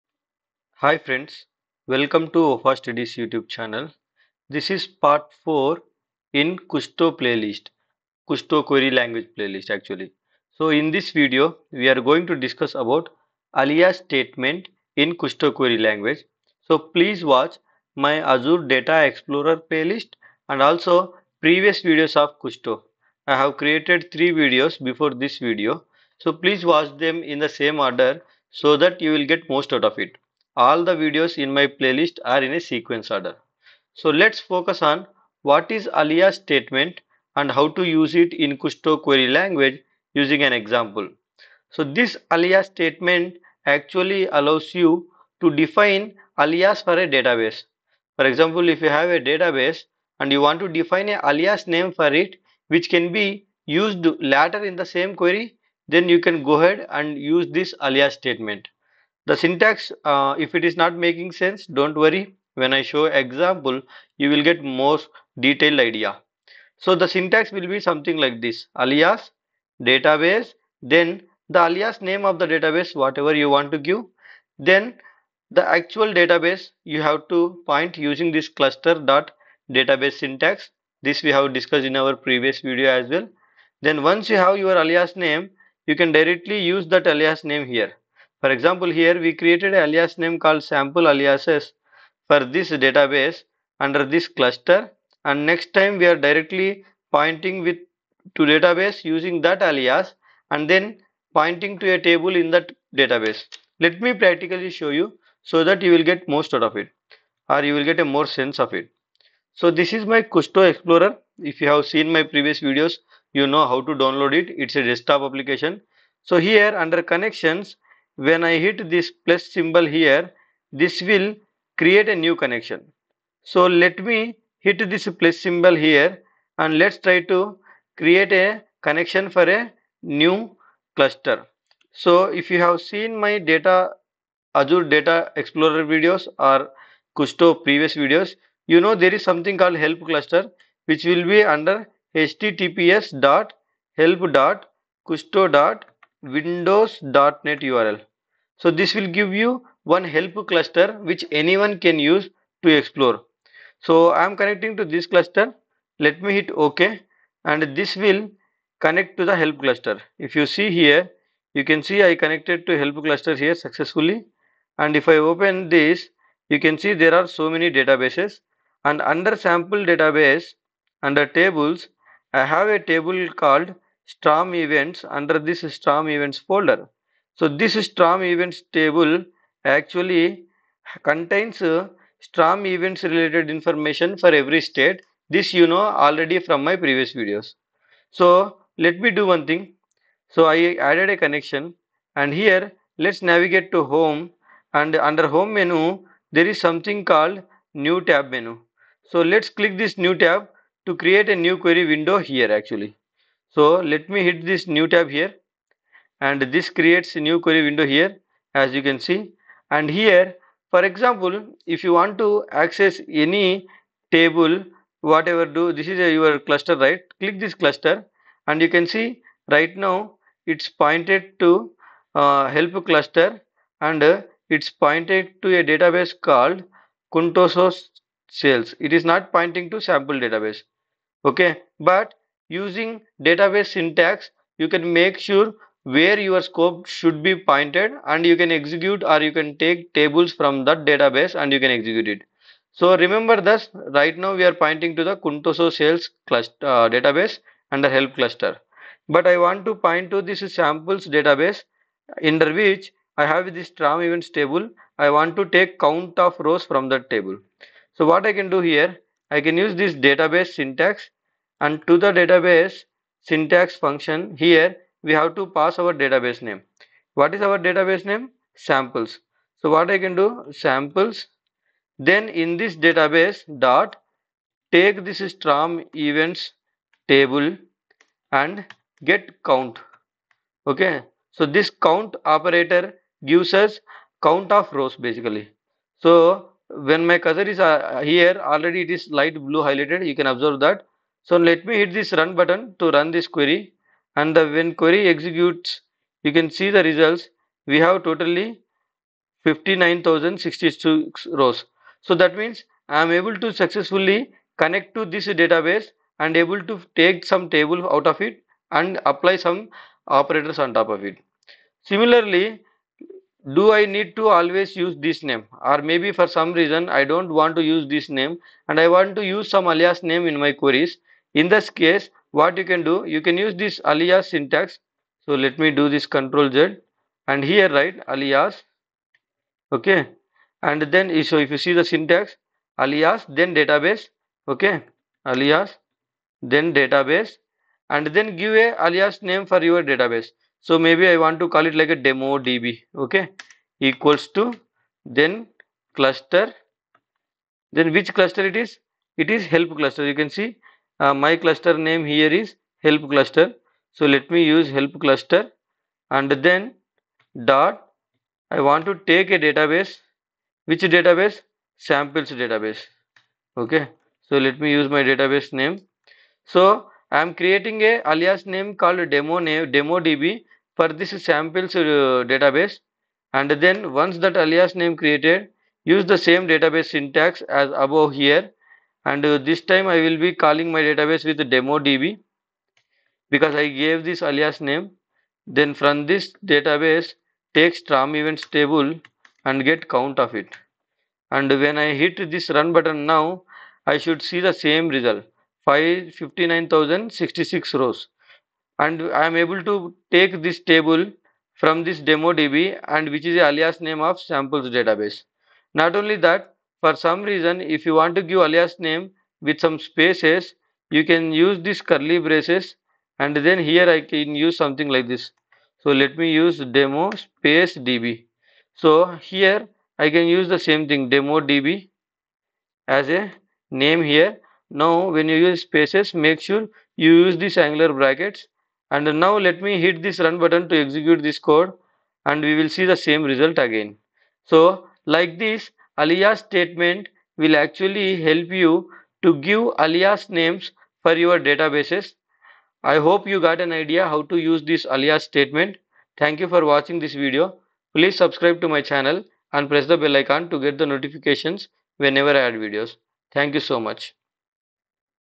4. Alias Statement in KQL
In this video, I discussed about alias statements in Kusto Query language and how to use it to give alias names for databases.